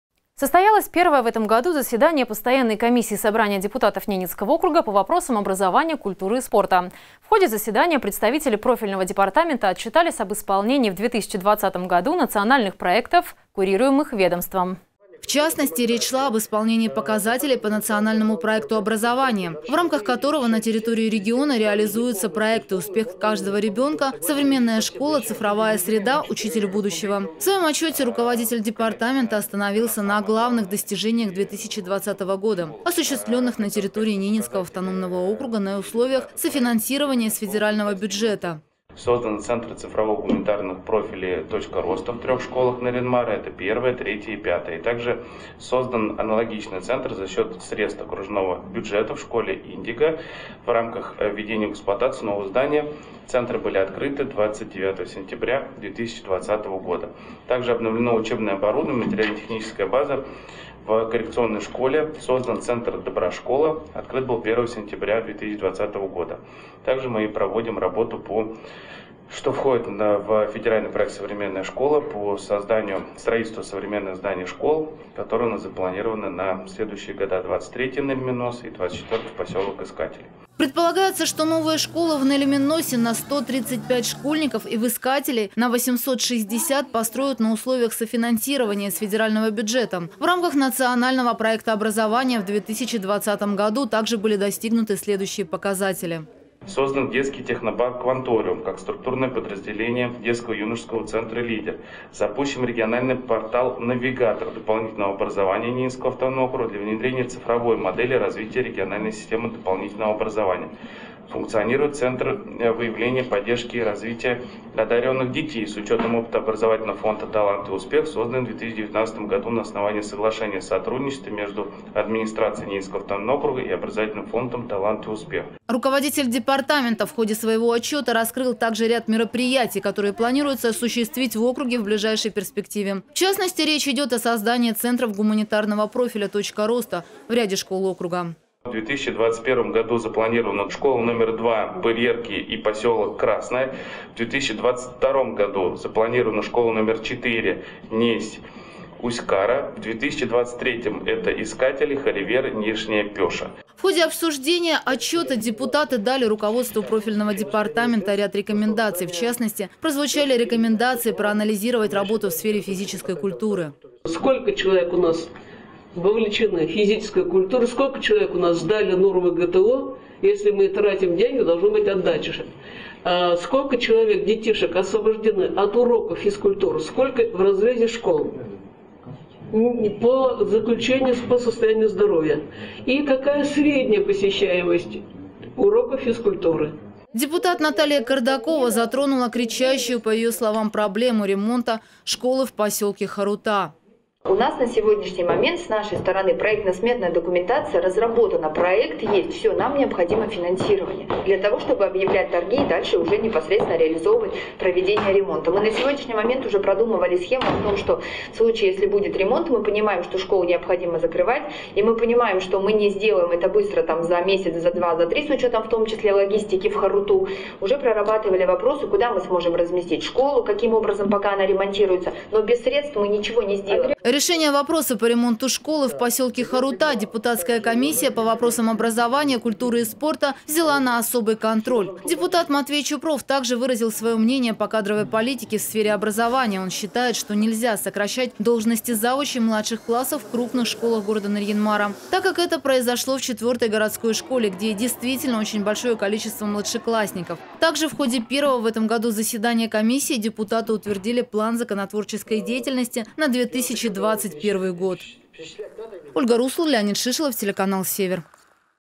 В ЦЕНТРЕ ВНИМАНИЯ - ОБРАЗОВАНИЕ
На первом заседании в этом году постоянной комиссии Собрания депутатов Ненецкого автономного округа по вопросам образования, культуры и спорта. В ходе заседания представители профильного департамента отчитались об исполнении в 2020 году национальных проектов, курируемых ведомством.